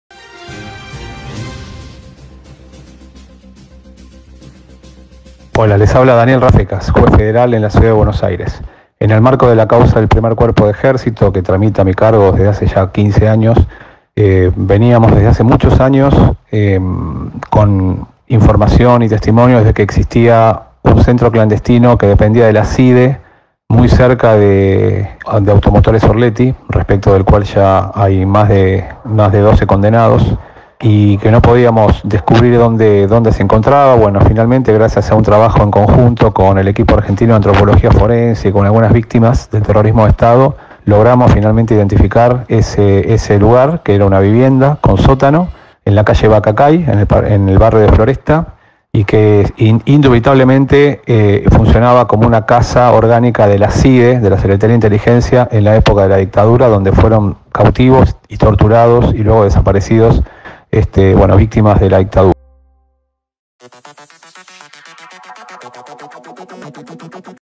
HUELLAS DEL GENOCIDIO
La Justicia Federal detectó otro centro clandestino de detención en base a los aportes que realizaron las víctimas, en uno de los juicios que se está llevando adelante. El lugar, está en la misma manzana en la que funcionó el centro "Automotores Orletti".